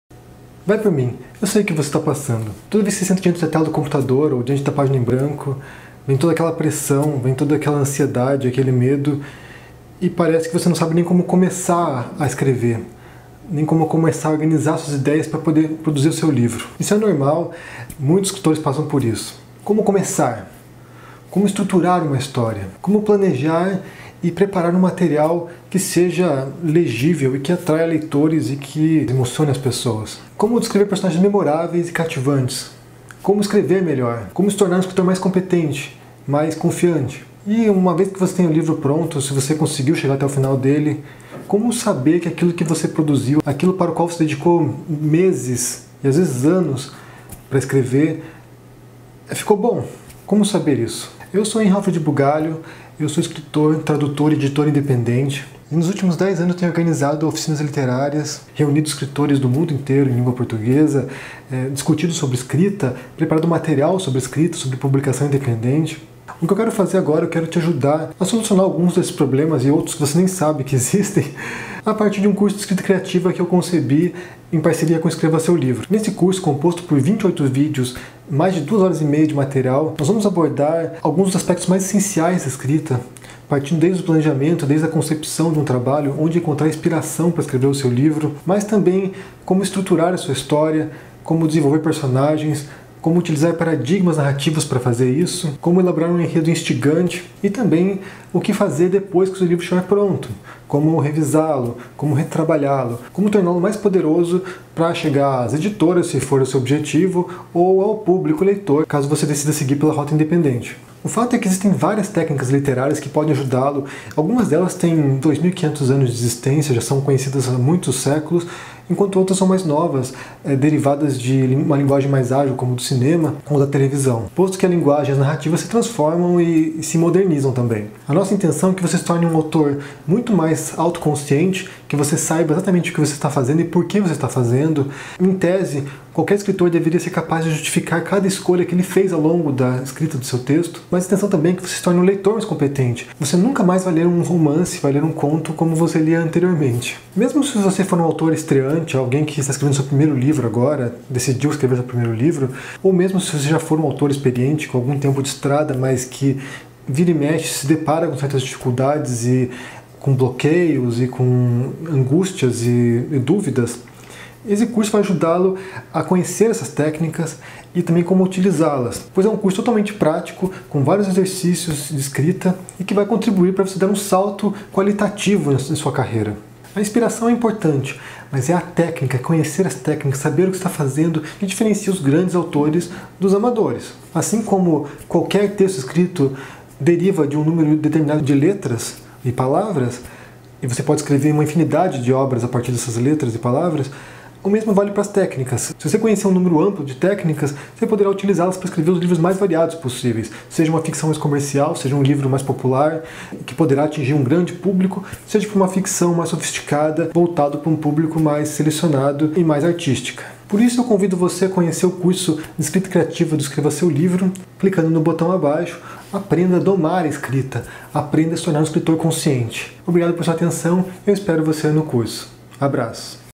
Curso Escrita Criativa - Escreva seu Livro | Apresentação do curso | Henry Bugalho
Curso Escrita Criativa do site Escreva seu Livro.
Aprenda com Henry Bugalho a aprimorar seus textos:

Aprenda a Planejar, Organizar e Executar as Ideias Para Escrever um Livro de Sucesso!

Descubra agora Como Pensam os Editores de Livros e Multiplique as Chances de Publicar sua Obra Sem Pagar Nada!

Apresentação Curso Escrita Criativa do site Escreva seu Livro, com Henry Alfred Bugalho

Vai por mim, eu sei o que você está passando.
Toda vez que você está sentado no computador e deixa tua página em branco, vem toda aquela pressão, vem toda aquela ansiedade, aquele medo, e parece que você não sabe nem como começar escrever e nem como começar a organizar suas ideias para produzir o seu livro.
Isso é normal. Muitos escritores passam por isso.
Como começar? Como estruturar uma história?
Como planejar e preparar um material que seja legível, que atraia leitores e que emocione as pessoas?
Como escrever histórias memoráveis e cativantes?
Como escrever melhor?
Como se tornar um escritor mais competente, mais confiante?
E, uma vez que você tem o livro pronto, se você conseguiu chegar até o final dele, como saber que aquilo que você produziu, aquilo no qual você dedicou meses, e às vezes, anos para escrever ficou bom?
Como saber isso?
Eu sou Henry Alfred Bugalho, eu sou escritor, tradutor e editor independente, e nos últimos 10 anos, eu tenho organizado oficinas literárias, tenho lido escritores do mundo inteiro em língua portuguesa, discutido sobre escrita, preparando material sobre escrita, sobre publicação independente, e o que eu quero agora é te ajudar a solucionar alguns problemas e outros que você nem sabe que existem, a partir de um curso de Escrita Criativa que eu concebi em parceria com o Escreva seu Livro.
Nesse curso composto por 28 vídeos de mais de duas horas e meia de material, nós vamos abordar alguns aspectos essenciais da escrita, partindo desde o planejamento até a concepção do trabalho, de encontrar inspiração para escrever o seu livro, mas também, como estruturar sua história, como desenvolver personagens, como usar paradigmas narrativos para fazer isso, como elaborar um enredo instigante, e também, o que fazer depois que o seu livro estiver pronto, como revisá-lo, como trabalhá-lo, como torná-lo mais poderoso para chegar às editoras, se for o seu objetivo ou ao público leitor, caso você queira seguir pela rota independente.
O fato é que existem várias técnicas literárias que podem ajudá-lo, algumas delas têm 2500 anos de existência, são conhecidas há muitos séculos, enquanto outras são mais novas, derivadas de uma linguagem mais fácil como do cinema ou da televisão, posto que as linguagens narrativas se transformam e se modernizam também.
A nossa intenção é que você se torne um autor muito mais autoconsciente, que você saiba o que está fazendo e o porquê de estar fazendo. Em tese, qualquer escritor deveria ser capaz de justificar qualquer escolha que ele fez ao longo da escrita do seu texto, mas para que você se torne, também, um leitor mais competente.
Você nunca mais vai ler um romance, um conto, como você lia anteriormente.
Mesmo se você for um autor estreante, alguém que está escrevendo seu primeiro livro agora, decidiu escrever seu primeiro livro, ou mesmo se você já for um autor experiente, com algum tempo de estrada, mas que vira e mexe, se depara com certas dificuldades e com bloqueios, angústias e dúvidas, o curso vai ajudá-lo a conhecer essas técnicas e também como utilizá-las, pois é um curso totalmente prático, com vários exercícios de escrita, o que vai contribuir para você dar um salto criativo na sua carreira.
A inspiração é importante, mas a técnica, saber o que está fazendo é o que diferencia os grandes autores dos amadores, assim como qualquer texto escrito deriva de um número determinado de letras e palavras, e você pode escrever uma infinidade de obras a partir dessas letras e palavras, o mesmo vale para as técnicas.
Se você conhecer um número amplo de técnicas, você poderá usá-las para escrever os livros mais variados possíveis, seja uma ficção mais comercial, seja um livro mais popular, que poderá atingir um grande público, seja uma ficção mais sofisticada, voltada para um público mais selecionado e mais artístico.
Por isso que eu convido você a conhecer o curso criativo do Escreva seu Livro clicando no botão abaixo.
Aprenda a domar a escrita, aprenda a se tornar um escritor consciente.
Obrigada por prestar atenção e eu espero você no curso! Abraço!